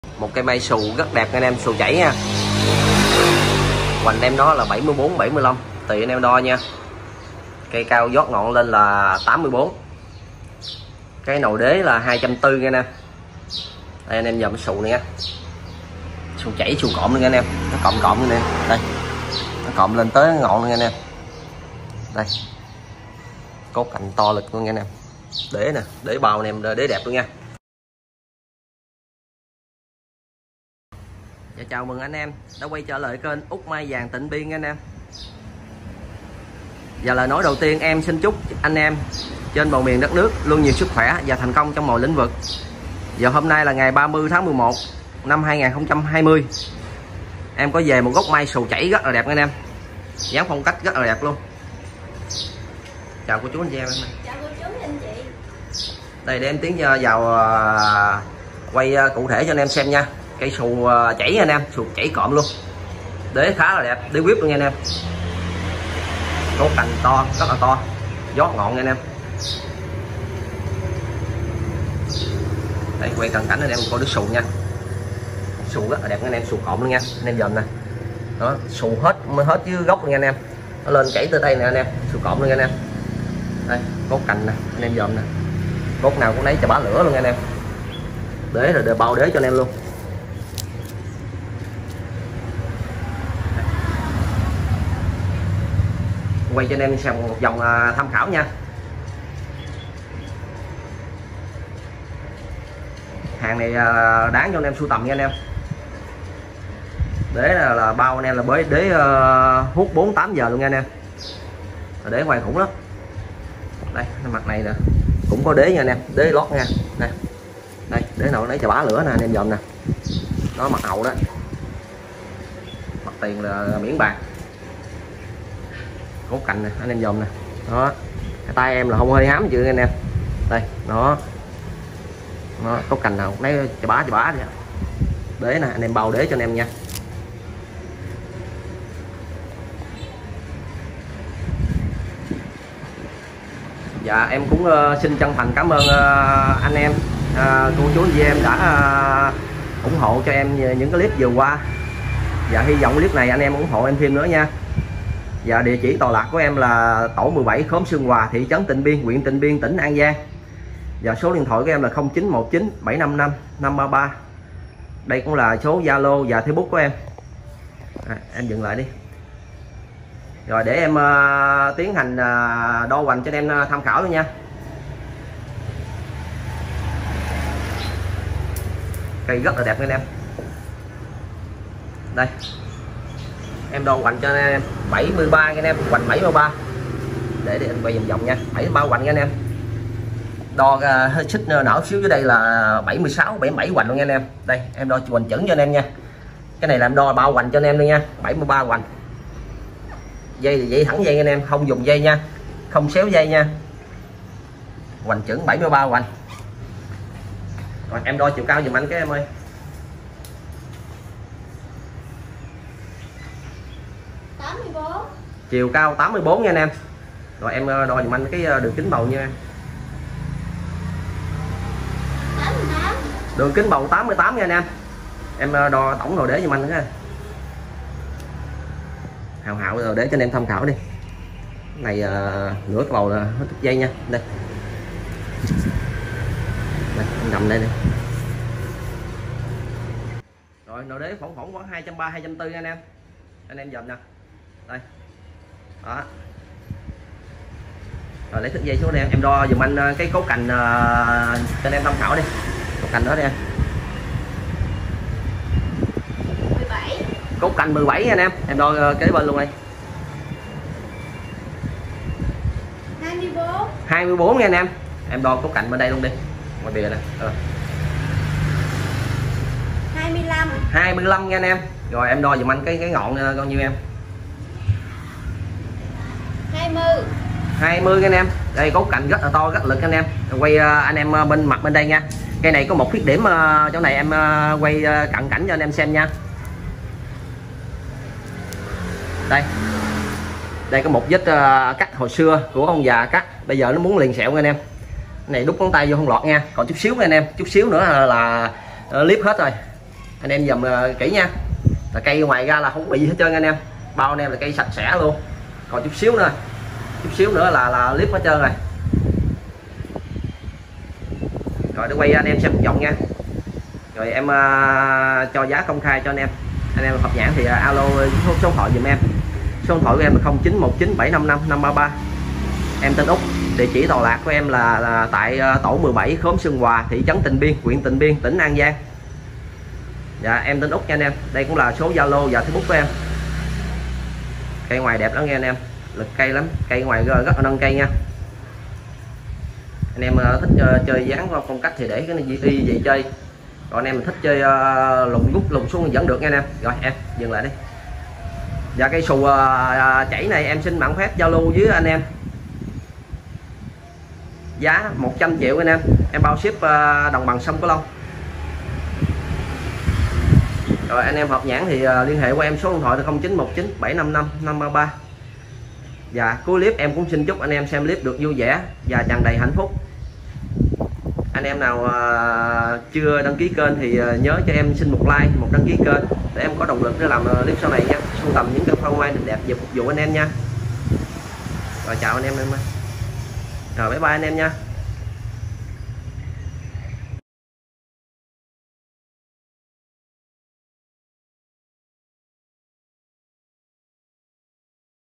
30/11/2020. Cây mai vàng xù chảy cốt cành siêu to siêu đẹp giá rẻ mềm nhe!lhệ: 0919755533 Út
Hôm nay xin giao lưu cùng anh em gần xa cây mai xù chảy cộm rất đẹp,cốt cành đều rất to lực vót ngọn rất đẹp! Cây nhìn ngoài mới phê mới đẹp hơn trong video nhe anh em! Rất mong anh em ủng hộ em tiếp tục nhé! ❤️
Chúc anh em xem video được vui vẻ hạnh phúc và thành công nhé!
Anh em cho em xin 1 like và 1 Đăng Ký Kênh cho em có động lực để sưu tầm những cây mai vàng đẹp để phục vụ cho anh em nhe ❤️